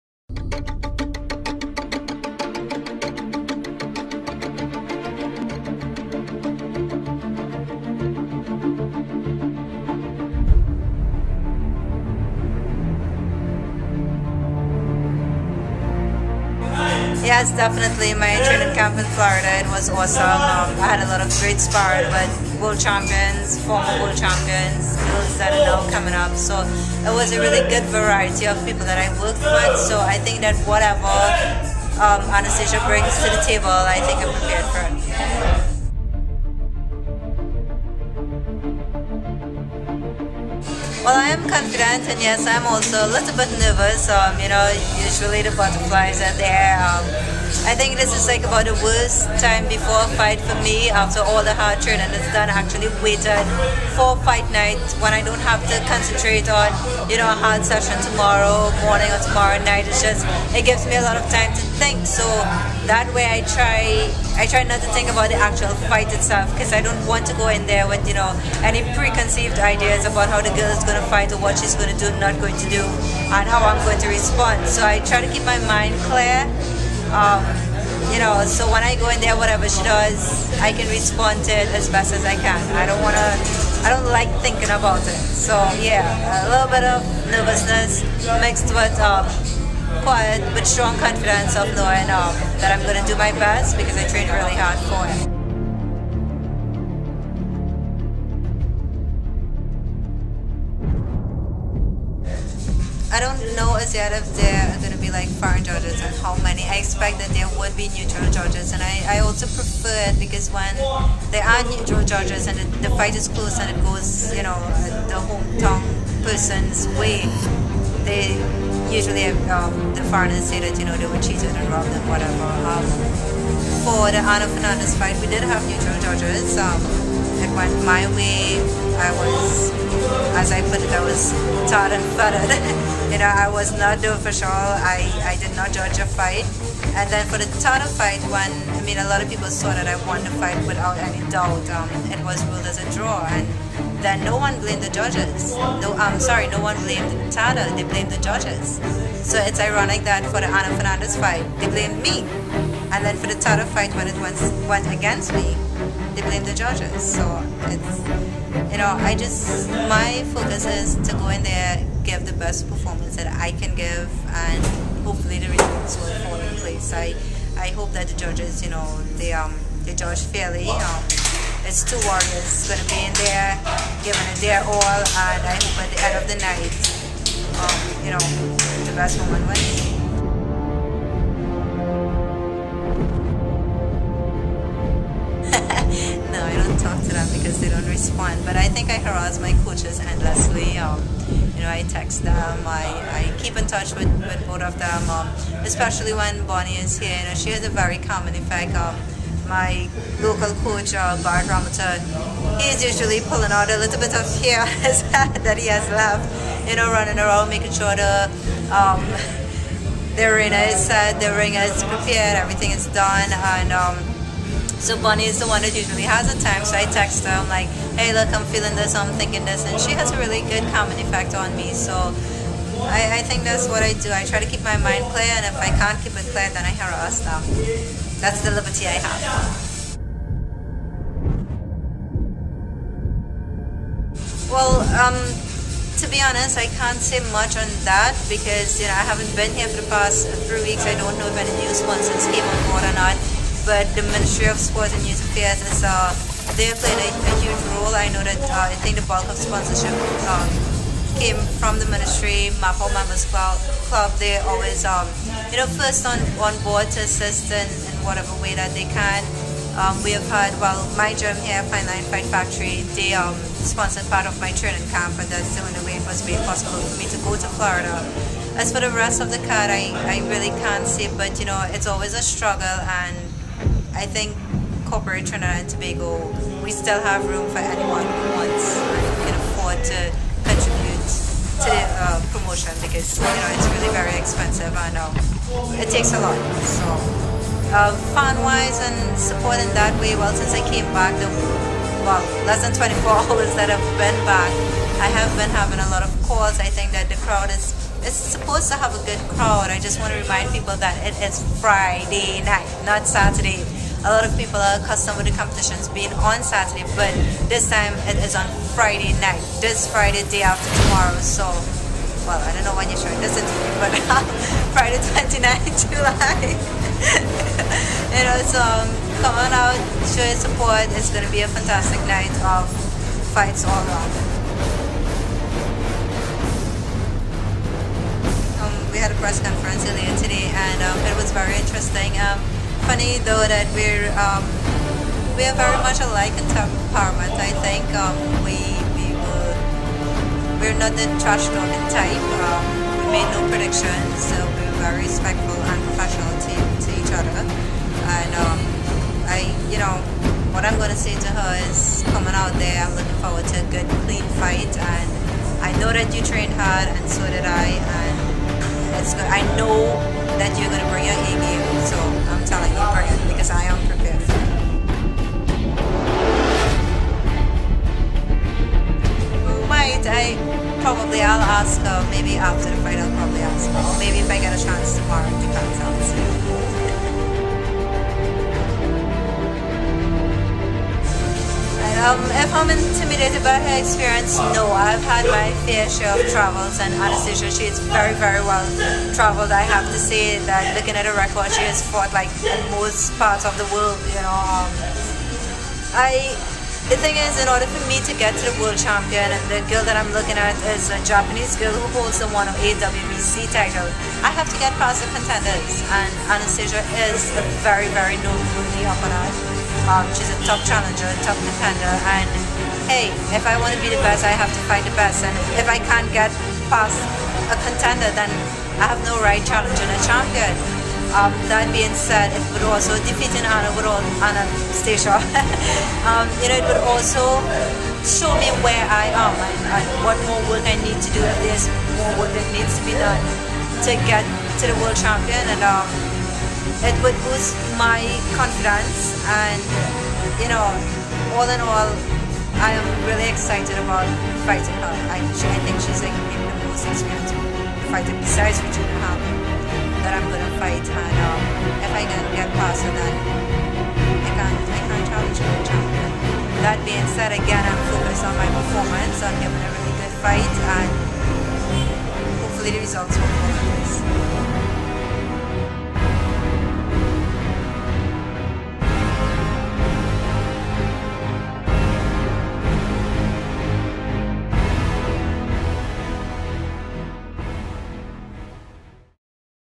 CWESN presents: Ria Ramnarine pre fight vs Anastasia Torktualova
Ria Ramnarine talks about her upcoming fight with Russian contender, Anastasia Torktualova.